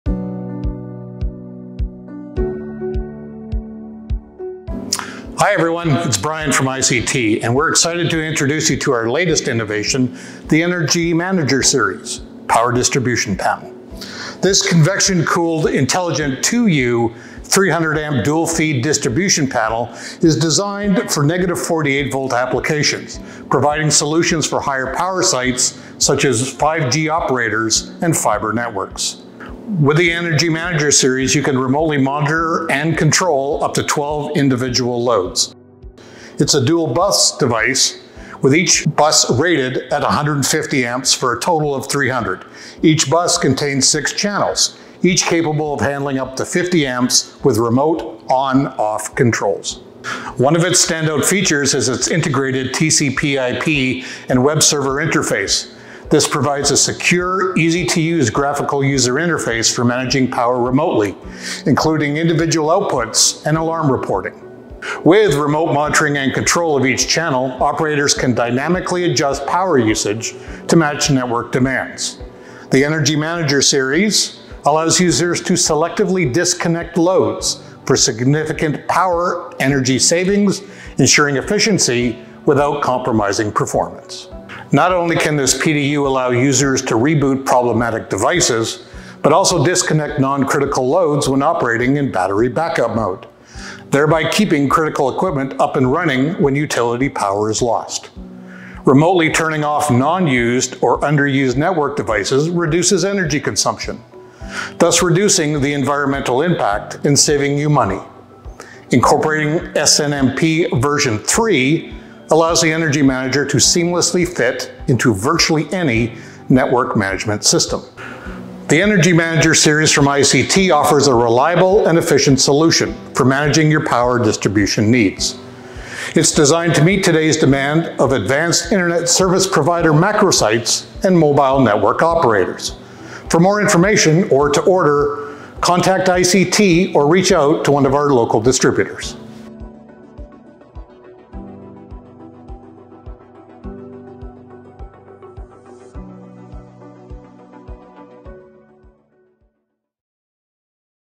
ICT's Energy Manager Series Overview
Watch Brian Kline, ICT's Major Accounts Manager, as he explains the benefits of ICT’s Energy Manager Series. This new solution features a 2U, 300-amp DC distribution panel designed to improve energy management and support high-power 5G networks. Learn how it drives efficiency and cost savings for your operations.